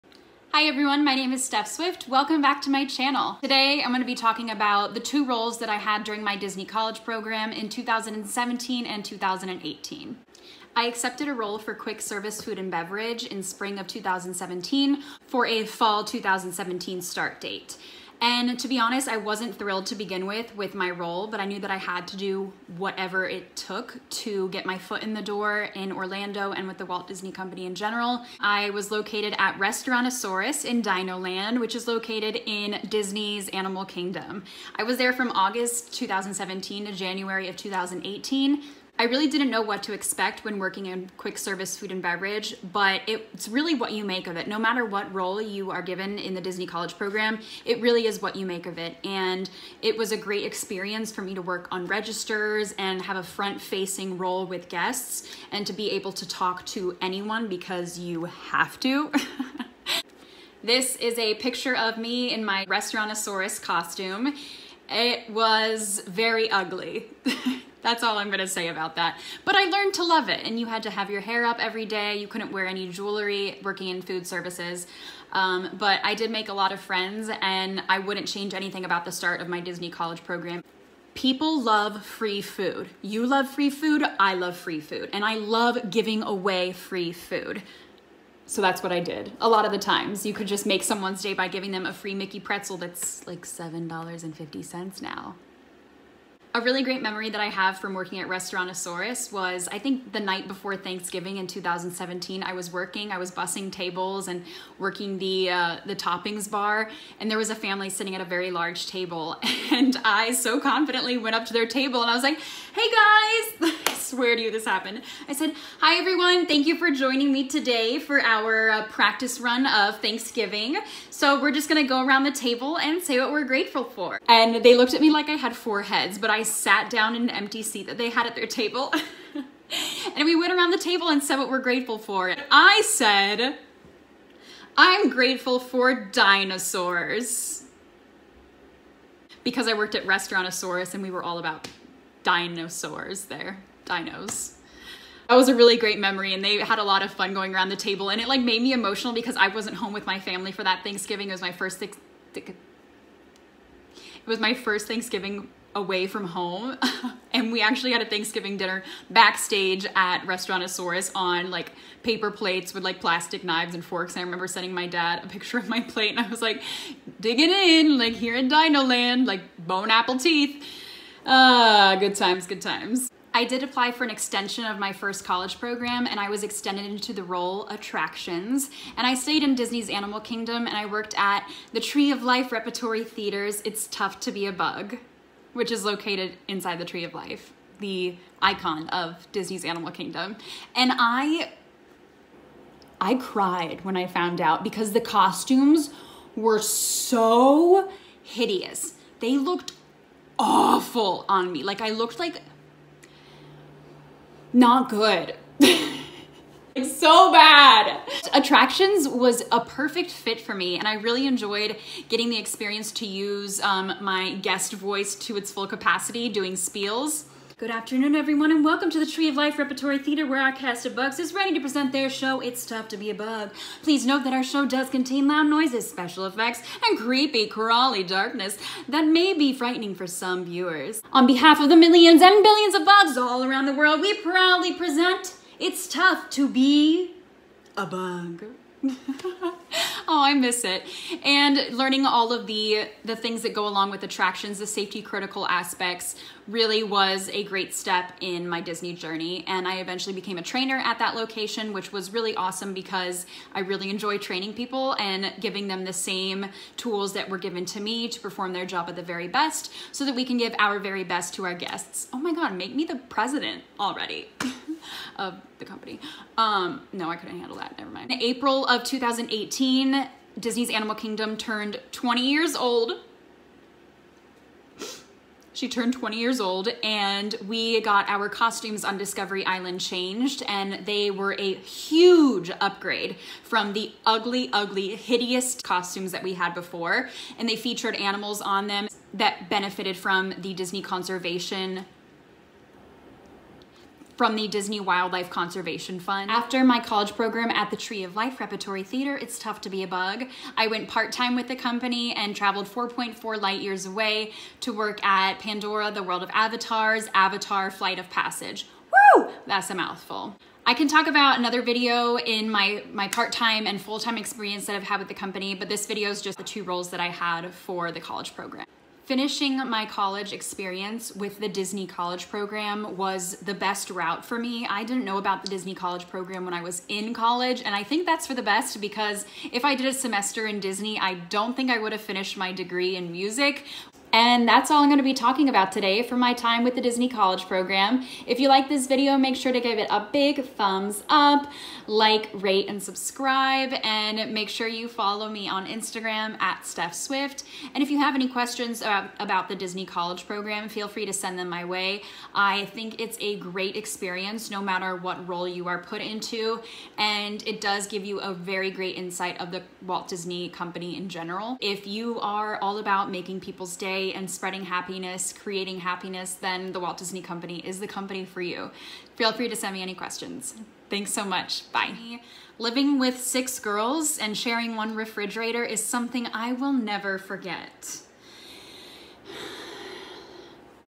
MY DISNEY COLLEGE PROGRAM
A little insight on my time with the Disney College Program!